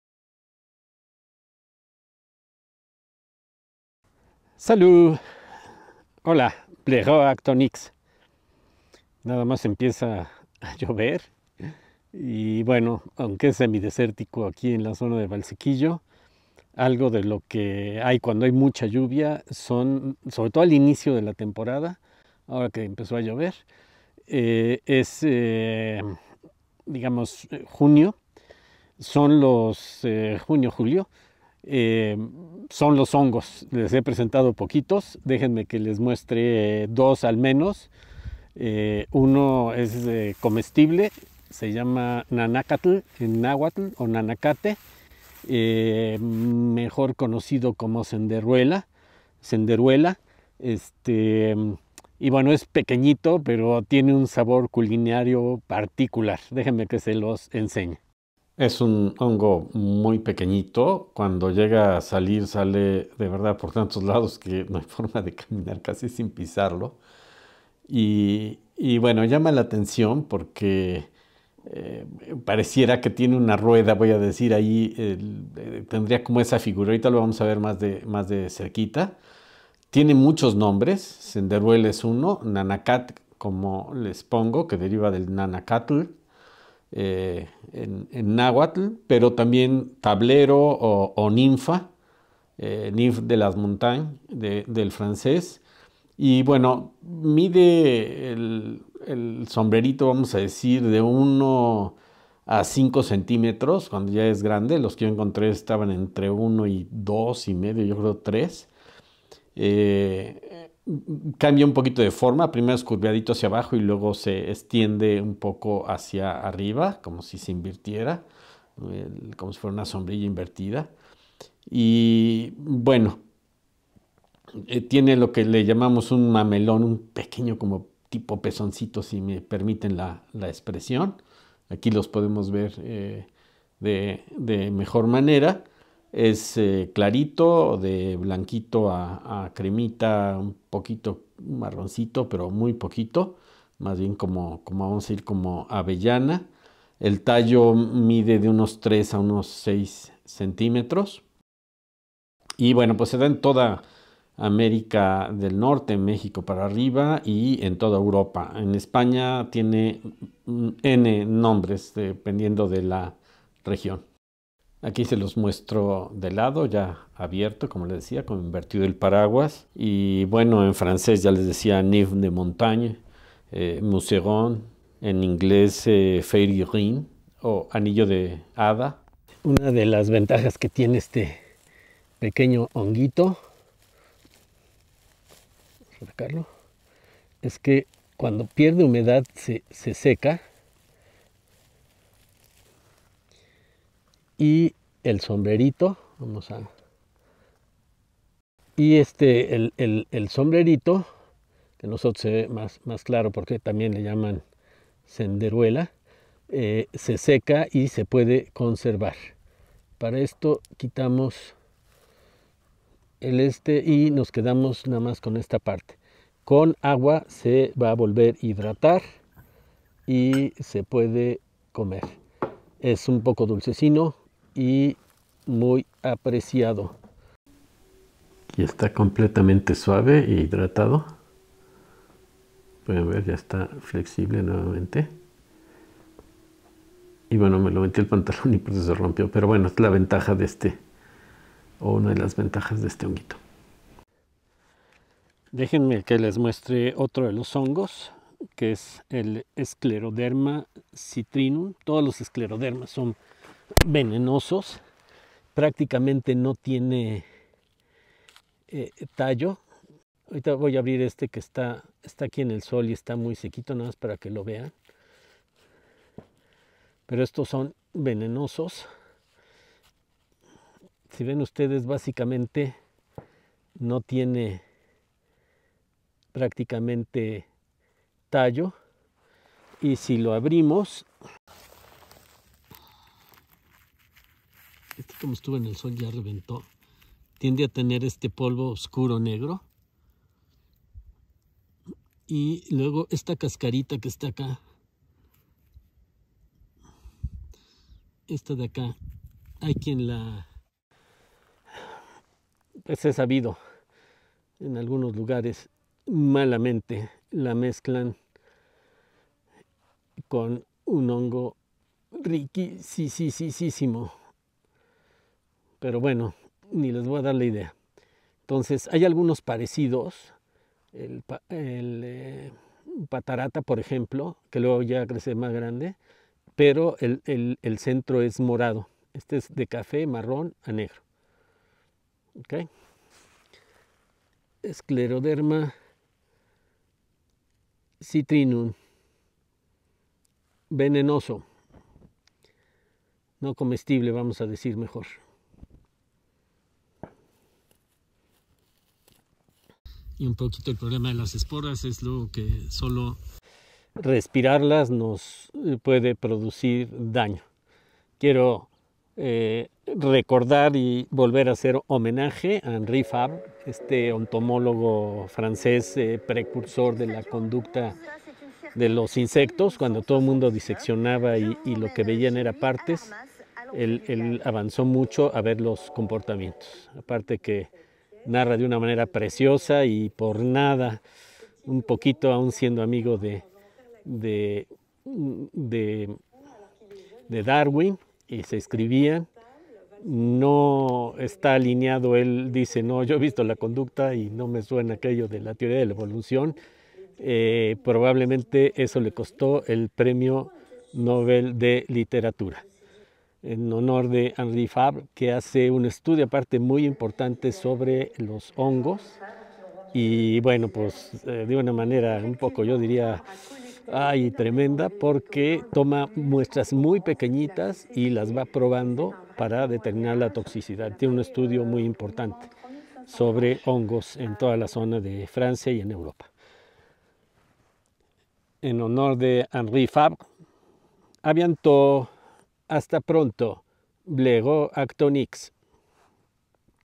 Valsequillo y sus hongos: Senderuela o Nanacate (Marasmius oreades) y Scleroderma citrinum
Te muestro dos hongos que crecen en la zona del lago de Valsequillo. Uno comestible conocido en México como Nanacate o Nanácatl en náhuatl y muy apreciado en España y otro que es tóxico, el Scleroderma citrinum